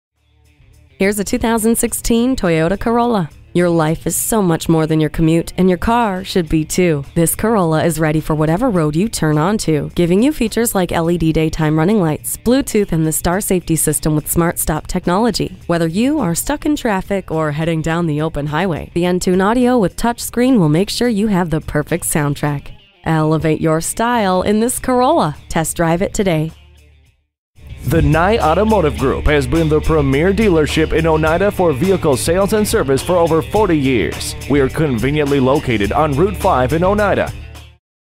2016 Toyota Corolla Utica NY Oneida, NY TT3232A - SOLD
Year: 2016
Make: Toyota
Model: Corolla
Trim: S
Engine: 4 Cyl. 1.8 L
Transmission: Variable
Color: Silver
Mileage: 26997
Stock #: TT3232A
VIN: 2T1BURHE6GC551938
T

Address:
1441 Genesee St.